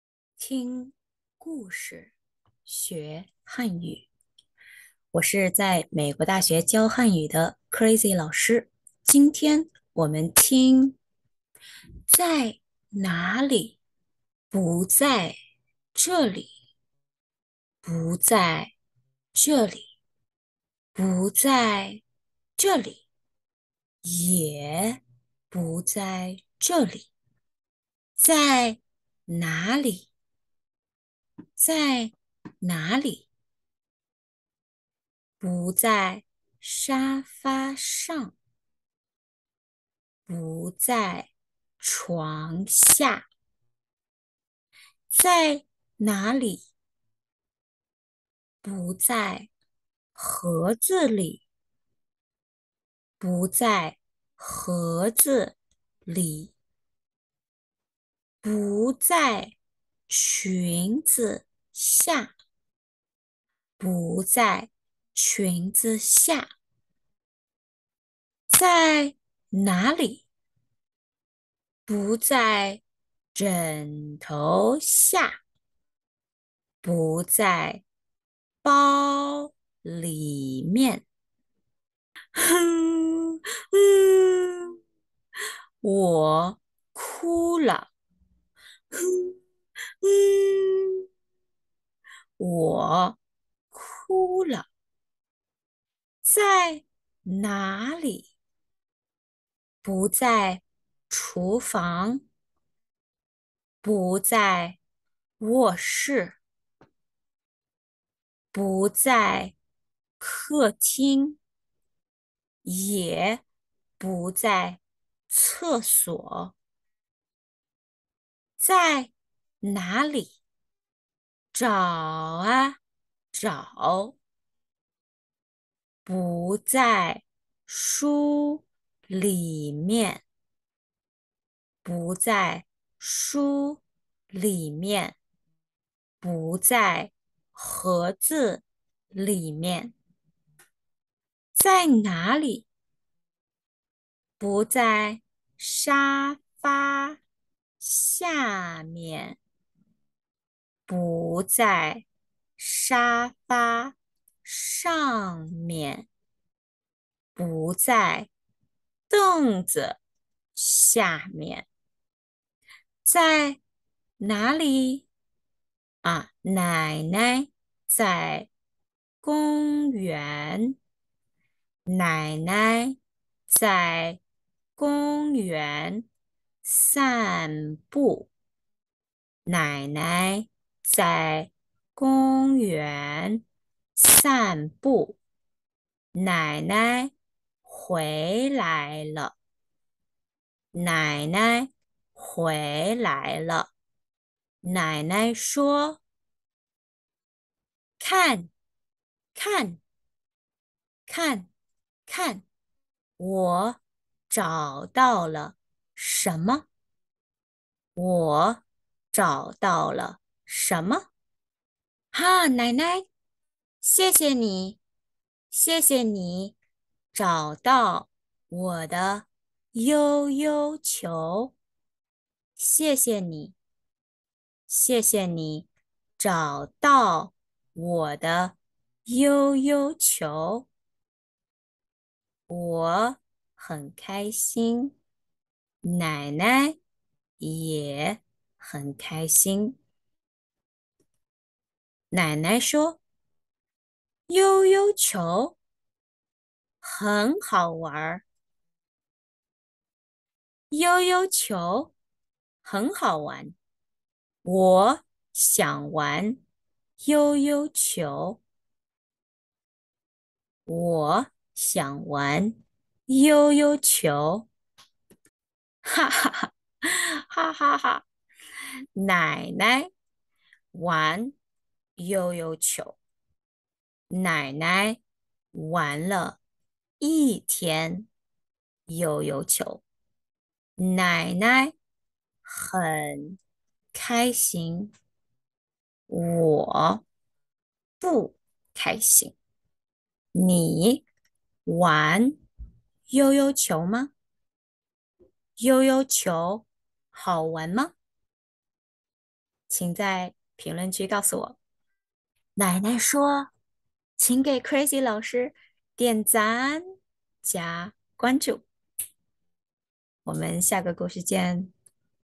Chinese Short Story for Beginners, Slow and Comprehensible Input在location words上面下面, Aquire HSK1
His favorite toy is gone. Where could it be? Listen to Easy Comprehensible Story and Improve Your Chinese naturally.

This series is designed for Real Beginners of Chinese. Try to understand through the context and picture. After you feel comfortable with the sounds, read along with the characters on the screen.

Author: Sukhada Rahalkar
Illustrator: Sukhada Rahalkar